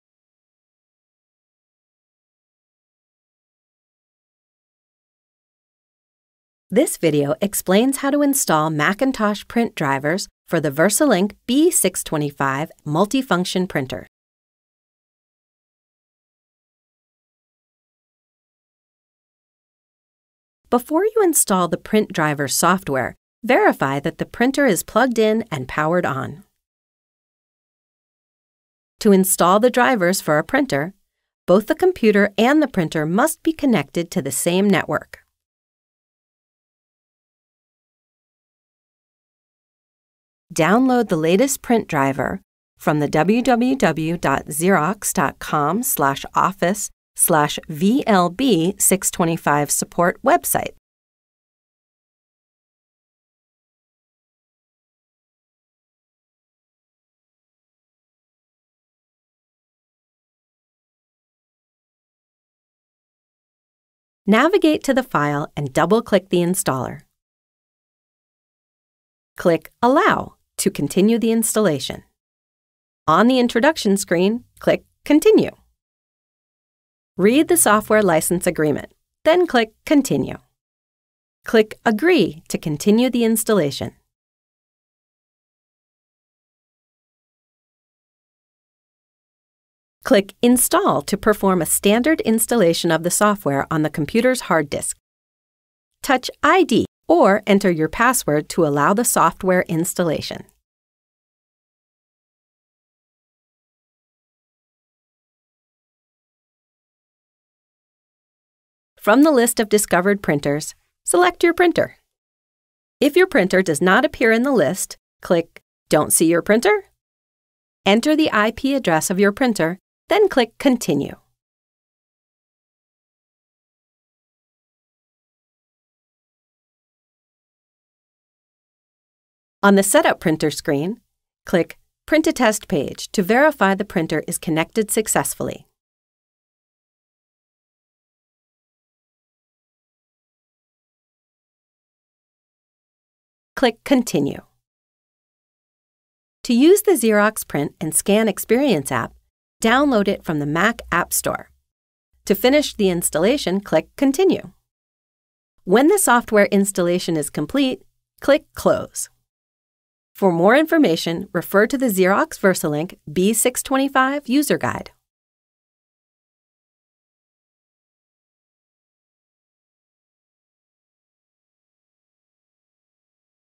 Xerox® VersaLink® B625 Multifunction Printer Install Macintosh Drivers
This video demonstrates how to download a Print Driver from Xerox.com and install the driver on a Macintosh computer.

BR38587